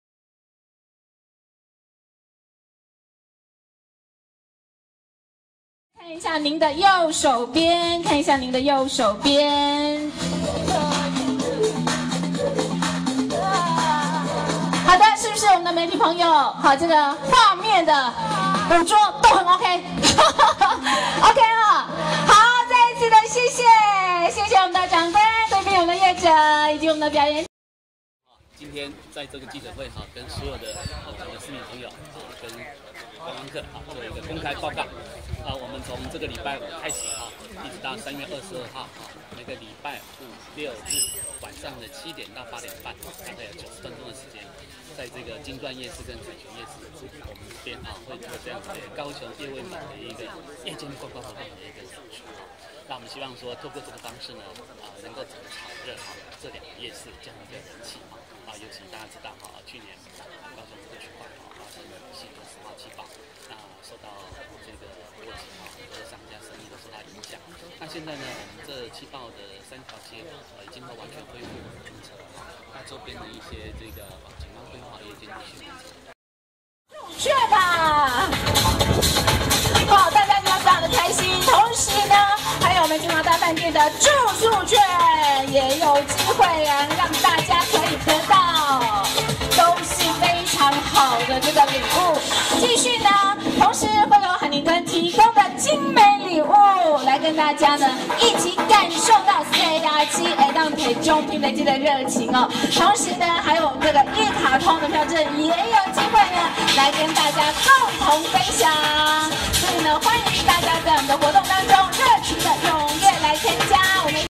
振興氣爆區觀光 夜市表演登場
（中央社記者程啟峰高雄3日電）高雄市觀光局為提振氣爆災區周邊觀光商機，選定在災區旁的金鑽夜市，6日起推出夜間觀光表演節目，吸引觀光人潮。